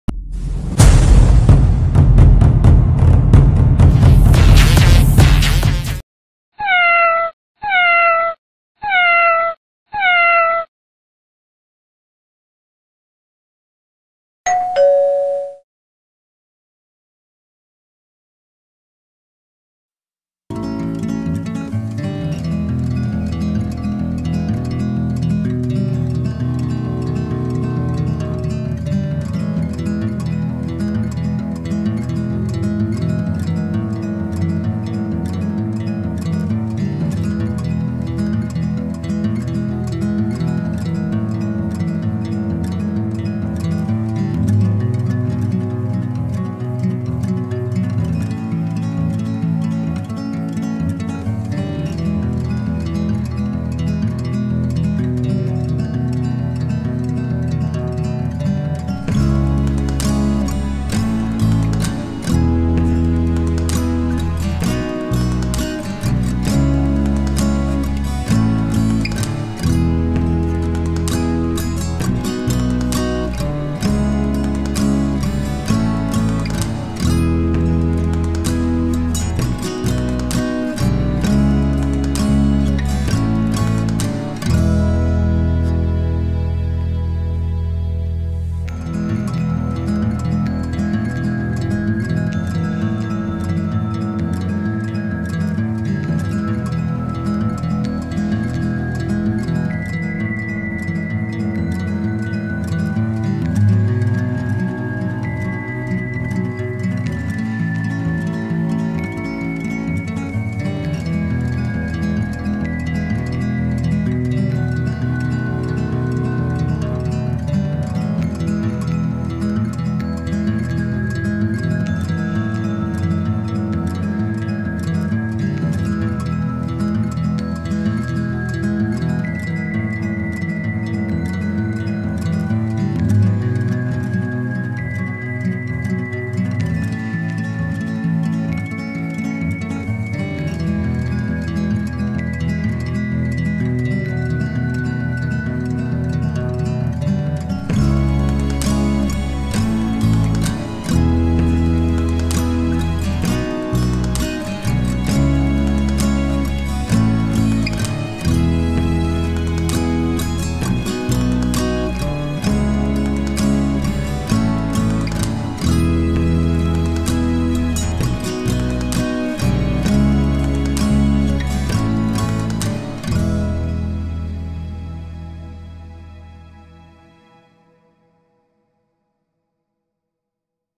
Cute Cat Cleaning Itself
A cute mother cat cleaning itself and playing with me.

Its also have two cute kittens